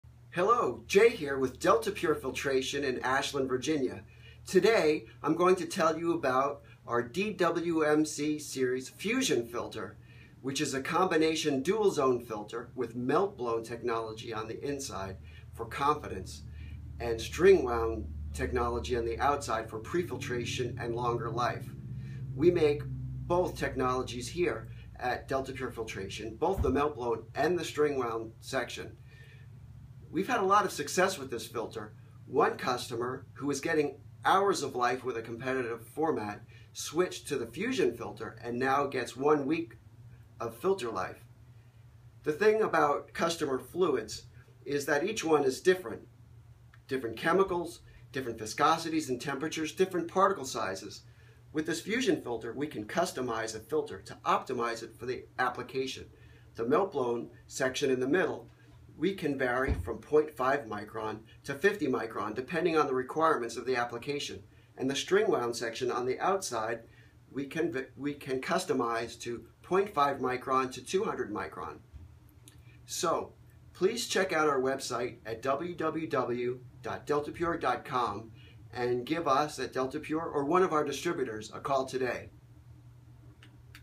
Fusion(TM) filter at Delta Pure Filtration
Find out more about Delta Pure and all our products

Jay with Delta Pure filtration talks about the Delta Pure Fusion(TM) filter with melt blown technology on the inside and string wound technology on the outside for pre-filtration and longer life. Manufactured in the USA at our Delta Pure Filtration facility.

This truly is a fantastic product. One of our clients told us that with a competitor's filter they had to change the filters every hour or so. With the Fusion(TM), they are able to get a week's life out of the the filter!